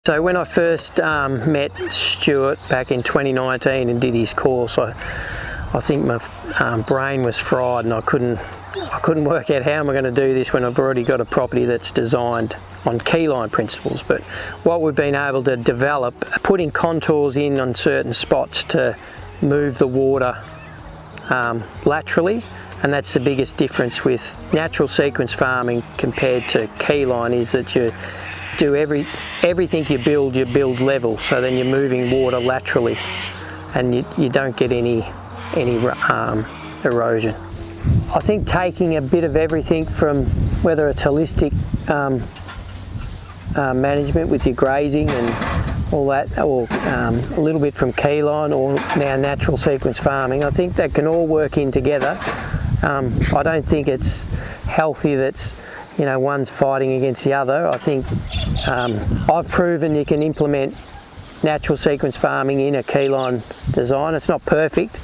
Keyline and Natural Sequence Farming: A Perfect Blend
Discover how natural sequence farming and Keyline design can work together for optimal land management. Despite initial doubts, adapting contours proves effective in moving water laterally and preventing erosion. 'It's not perfect,' but it works!